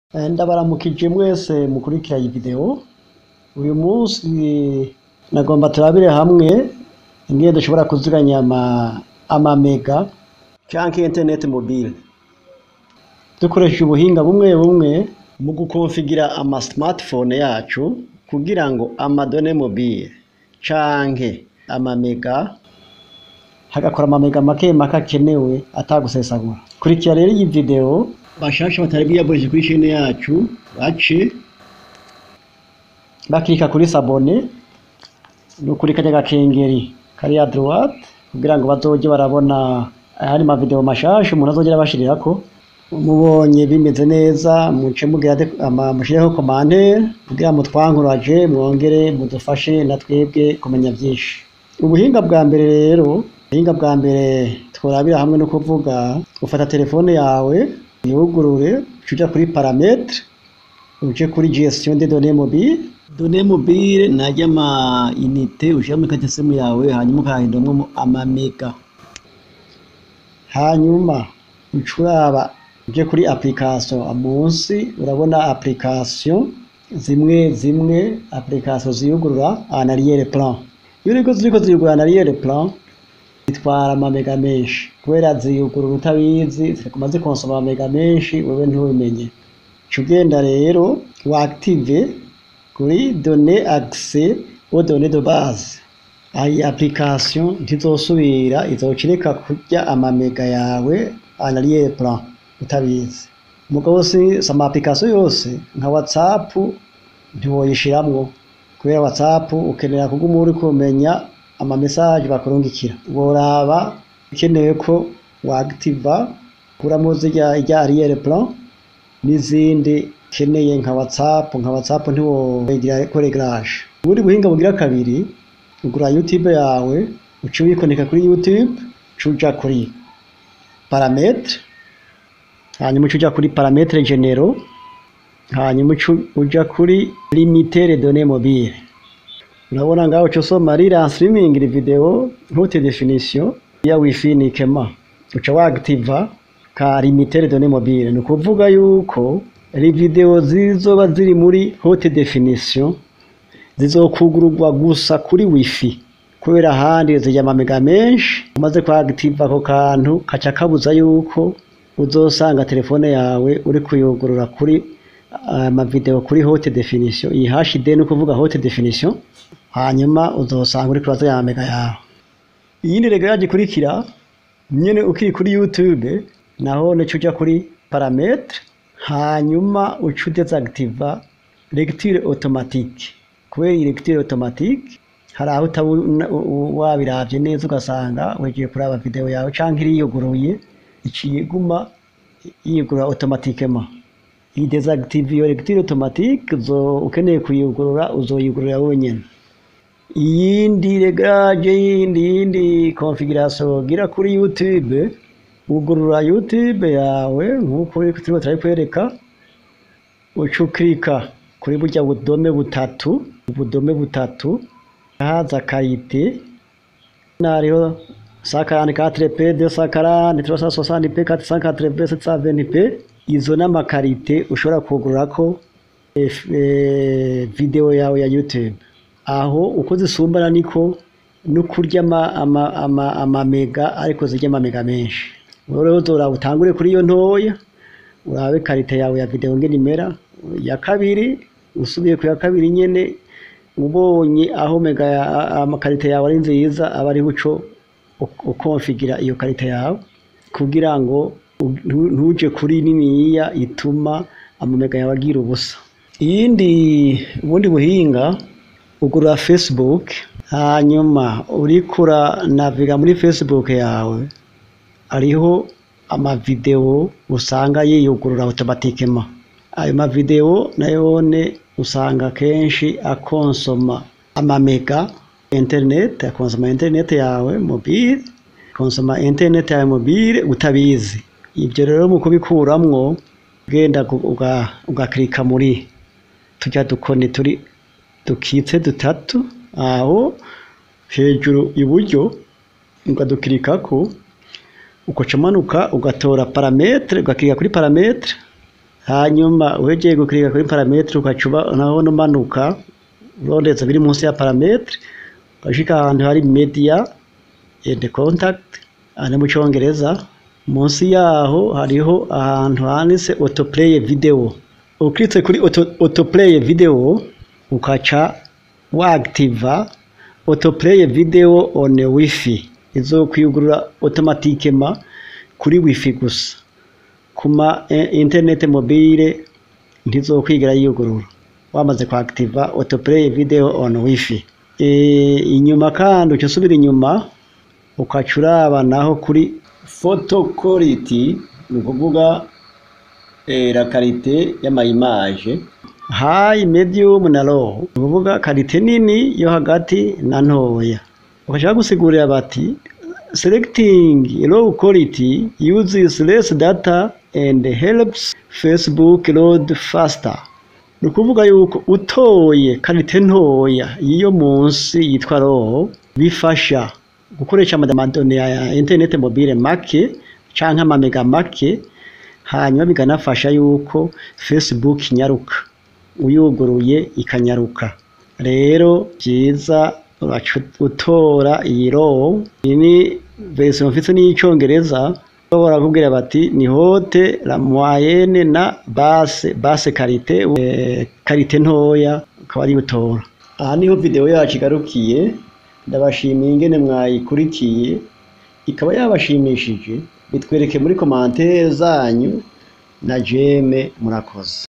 [TUTO 10] UBUHINGA BWO KUZIGANYA INTERNET MOBILE KURI PHONE ZANYU/ECONOMISER VOS MEGGAS / ANDROID
Bonjour,
L'objectif y'iyi vidéo nukwerekana uburyo butanu bubafasha gukoresha neza ama données mobiles canke internet mobile kuri smartphones zanyu. Iyi vidéo ibereka ubuhinga butanu bwo kuregera canke gu configira phones zanyu bigatuma am données mobiles yanyu atihuta guhera. Kuri bashasha mutari bwugurure chaine yacu twobasaba kwiyabonesha hanyuma ama commentaires yanyu yodufasha. Murakoze